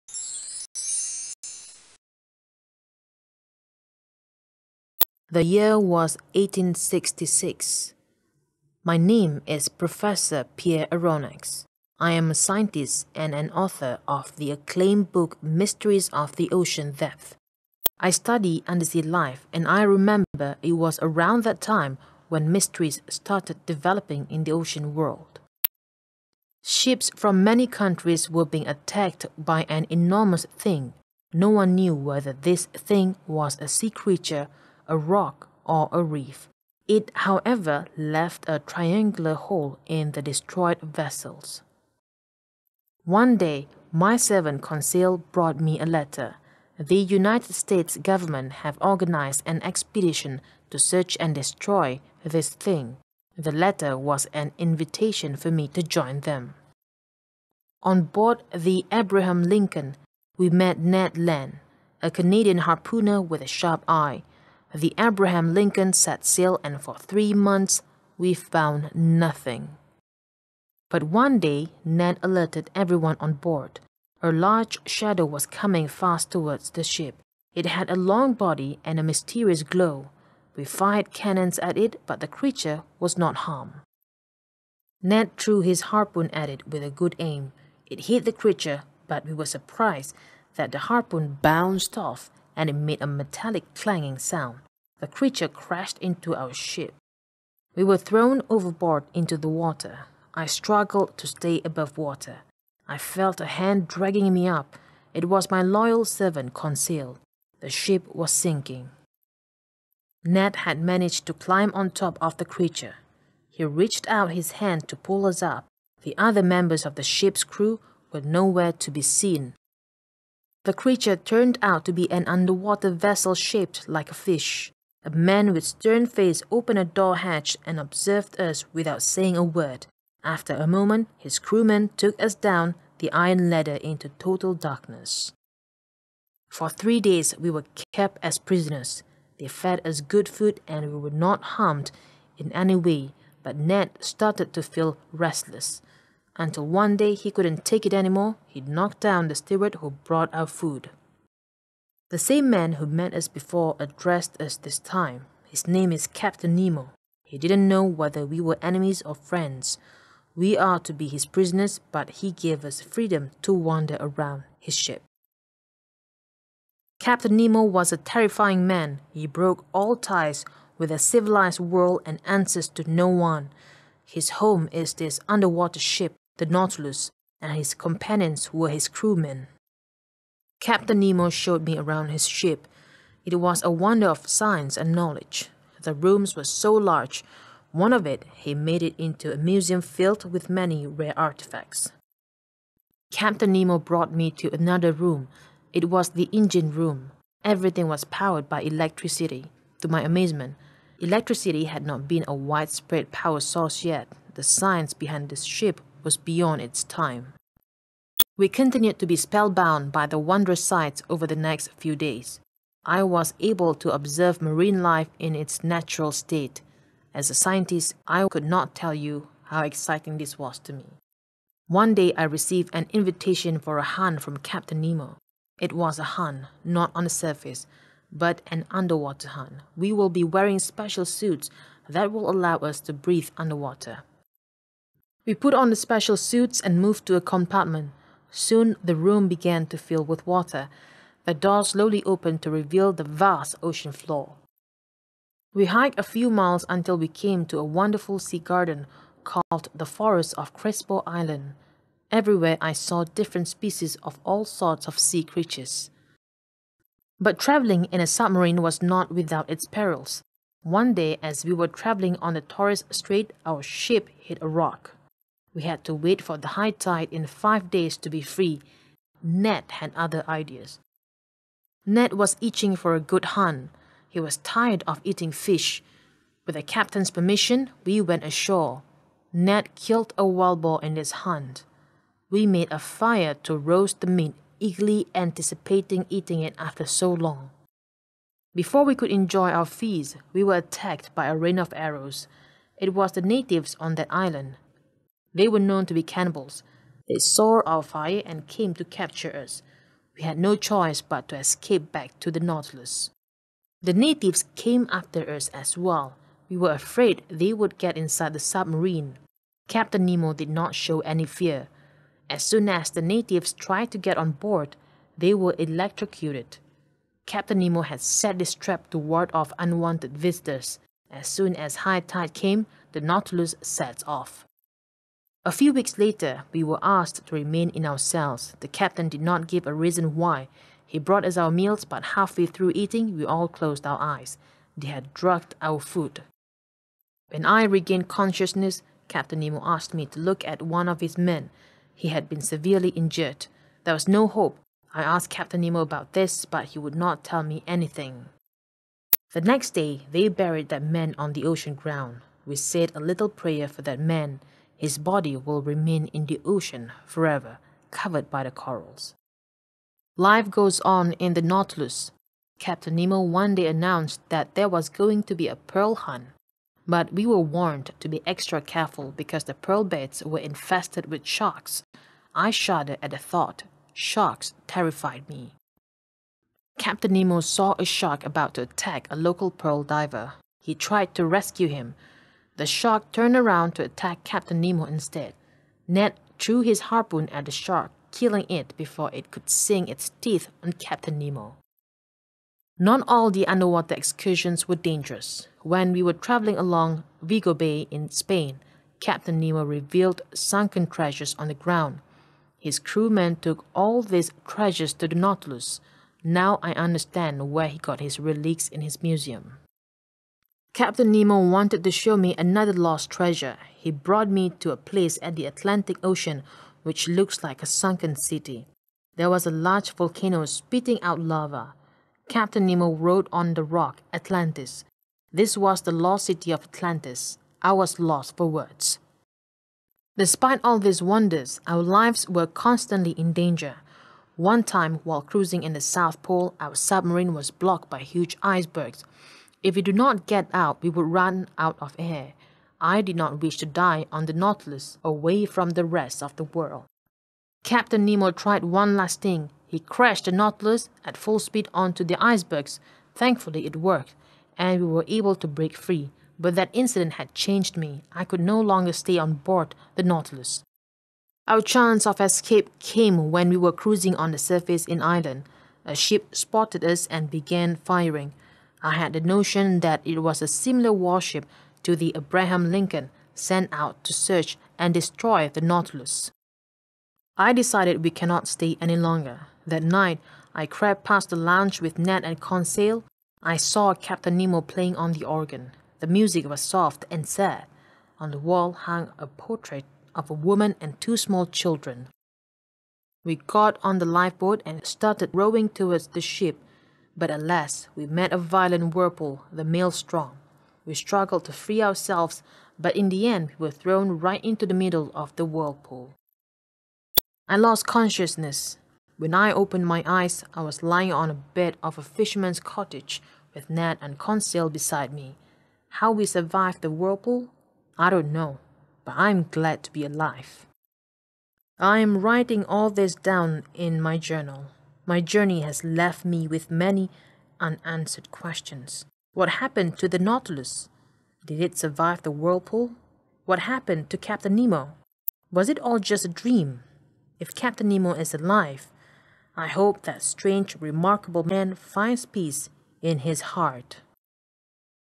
Novel: 20,000 Leagues Under the Sea
Textbook: 20,000 Leagues Under the Sea
Novel in Animation
Design by Myteach Sdn Bhd
Product in PowerPoint, ATM (Advance Teaching Module) and printed book, OMG (Oh My Guru) English